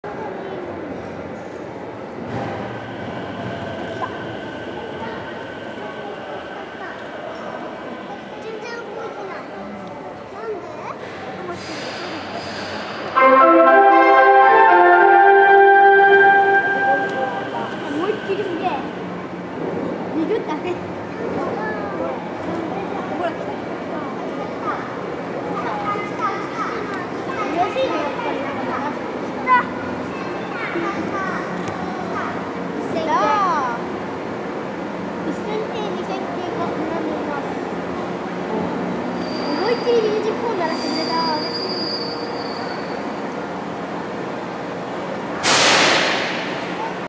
第7回つくばエクスプレスまつり2011 検修庫に到着するTX1000系
ミュージックホーンを鳴らして検修庫に到着するTX1000系です。